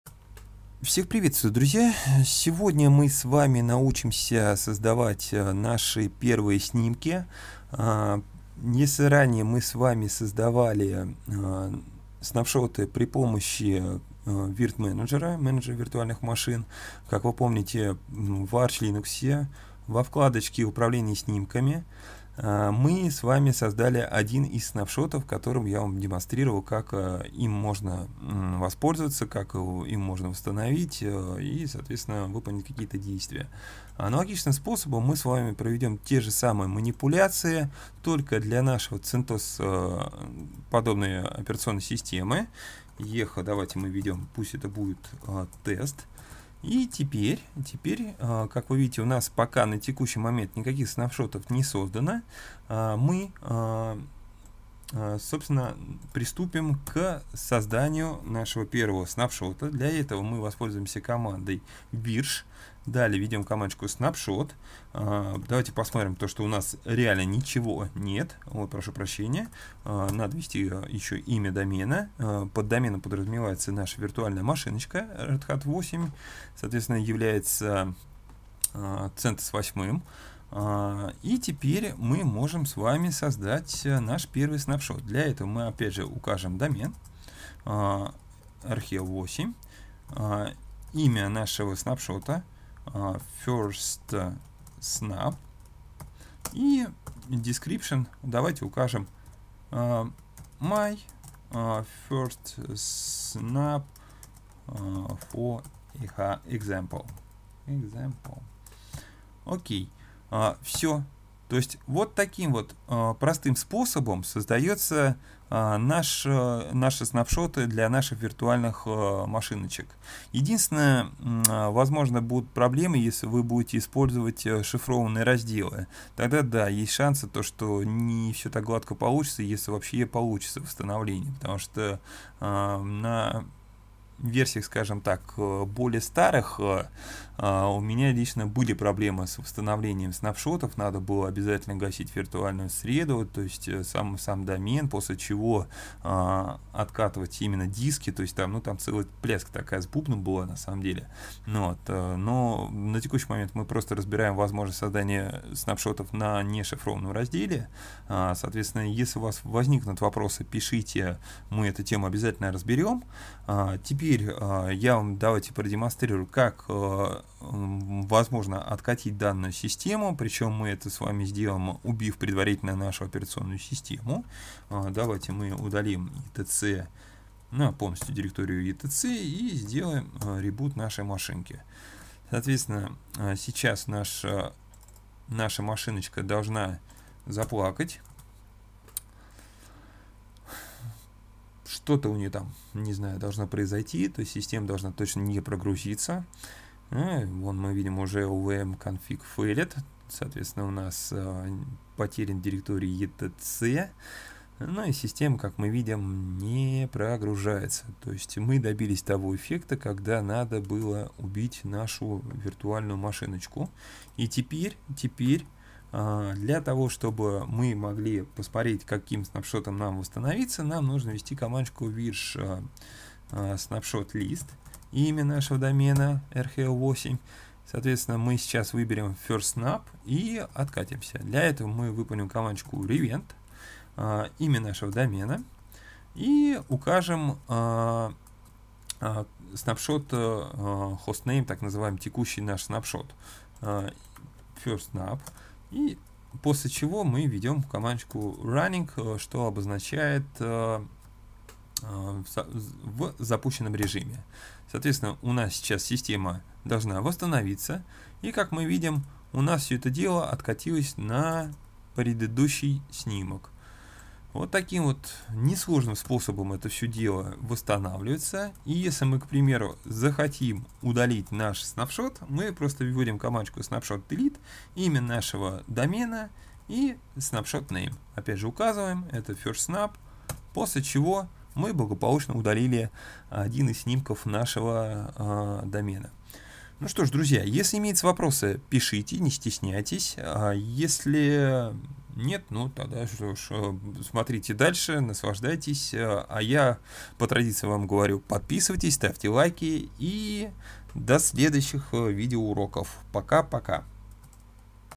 libvirt | virsh snapshot - работа со снимками create | revent | delete | list
Научимся работать со снимками в libvirt, для этого мы воспользуемся командой virsh snapshot-create, а так же будем откатывать snapshot-revent и создавать новые snapshot, удалять snapshot-delete.